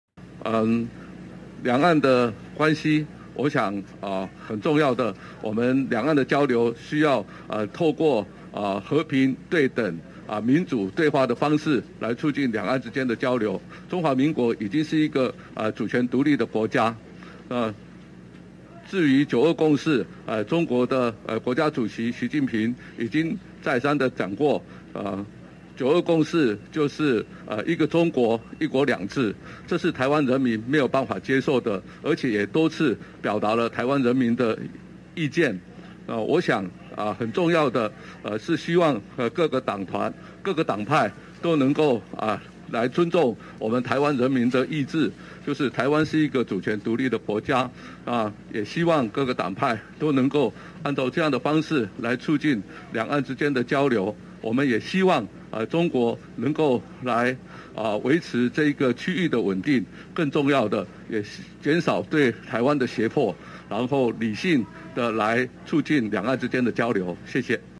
「台灣是主權獨立國家」 陳建仁盼各黨尊重人民意志【最新快訊】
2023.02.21【最新快訊】國民黨副主席夏立言召開記者會，大談訪中成果，認為九二共識的基本精神就是「一中各表」，對此行政院長陳建仁今（21）受訪表示，「中國國家主席習近平已經再三強調，九二共識就是一個中國、一國兩制，這是台灣人民沒有辦法接受的」。